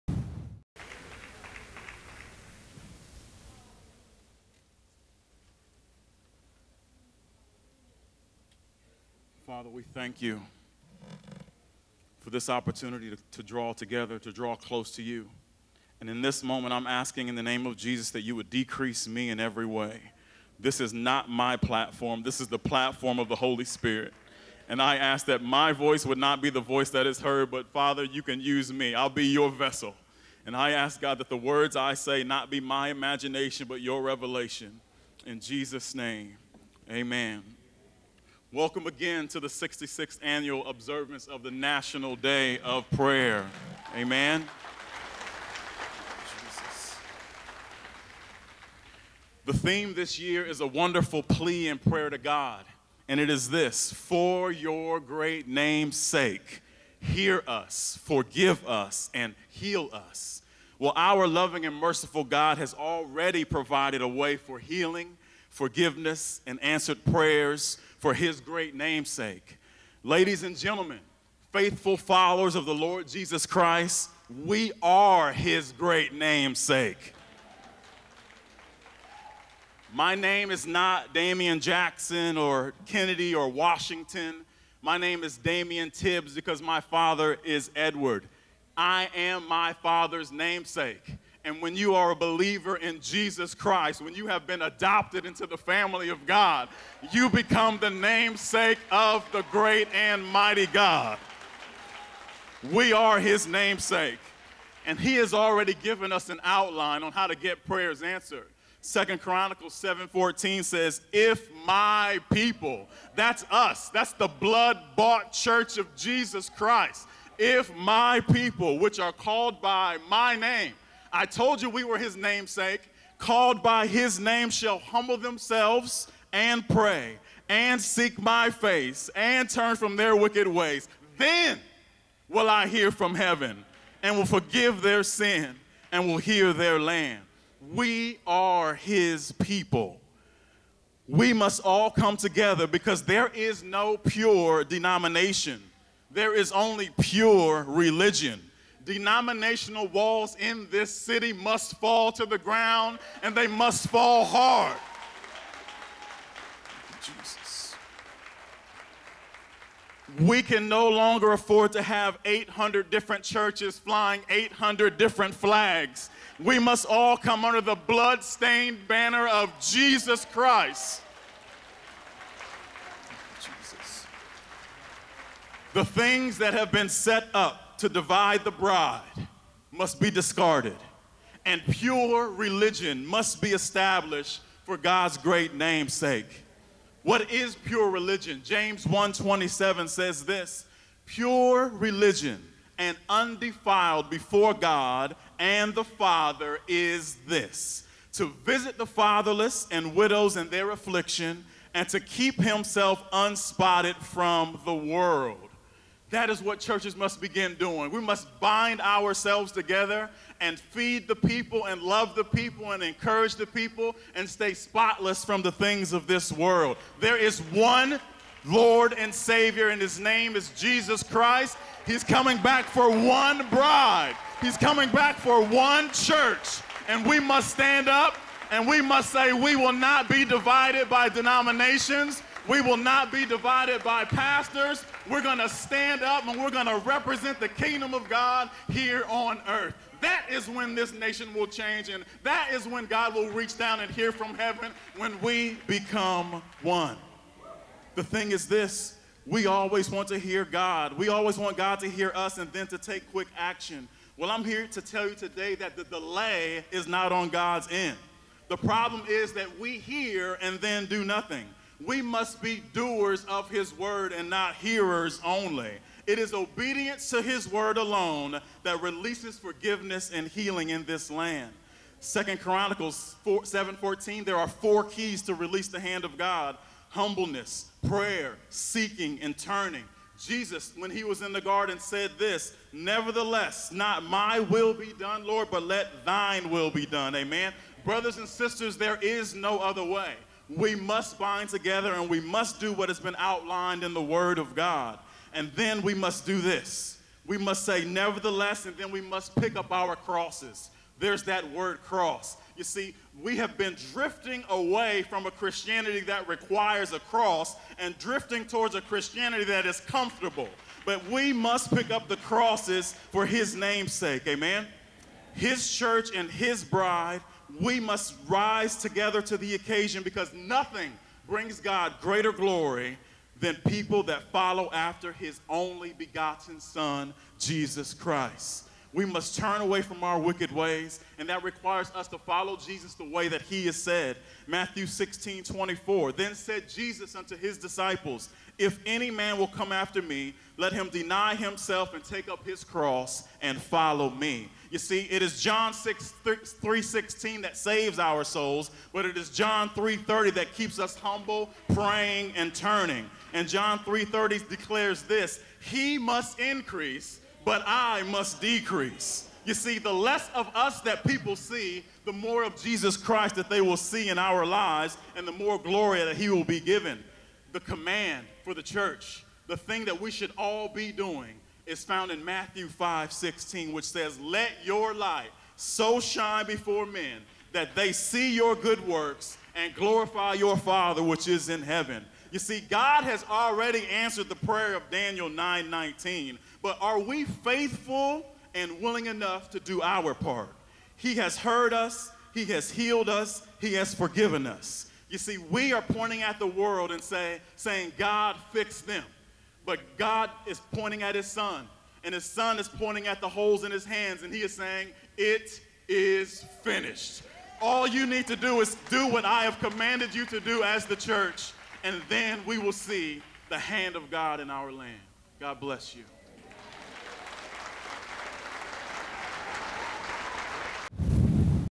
Damian Tibbs Speaks at the National Day of Prayer, Allen Co. OH 2017
Damian Tibbs is pastor of New Life Christian Ministries and a Deputy Sheriff with the Allen County Sheriff's Office in Lima, OH.
Pastor Tibbs powerful National Day of Prayer message creates a call to action- challenging the area churches to truly embrace the definition and creation of unity, in God's eyes.